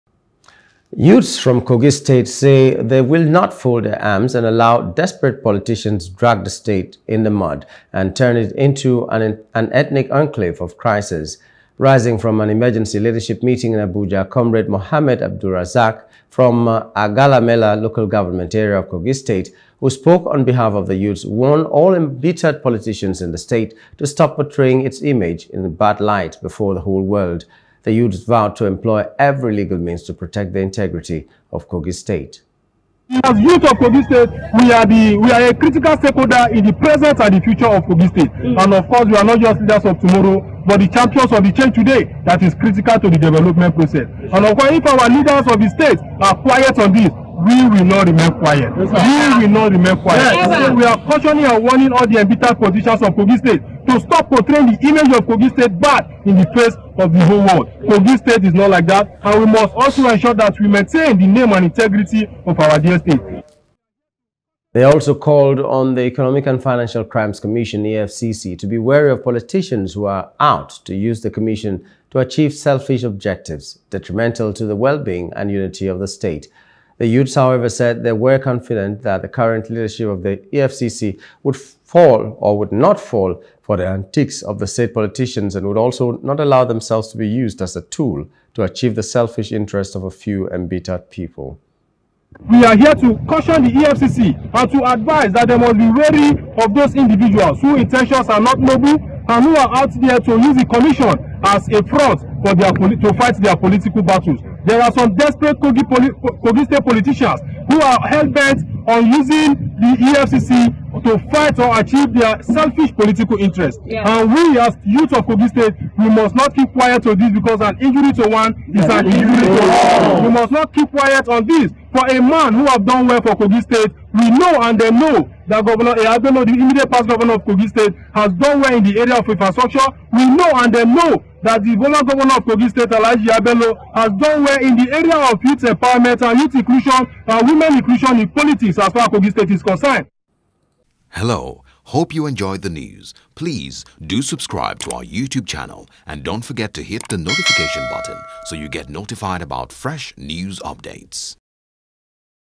Kogi Gov’ship Election Aftermath: Youths Warn Desperate Politicians Against Overheating Polity
Youths from Kogi State say they will not fold their arms and allow desperate politicians to drag the state in the mud and turn it into an ethnic enclave of crisis.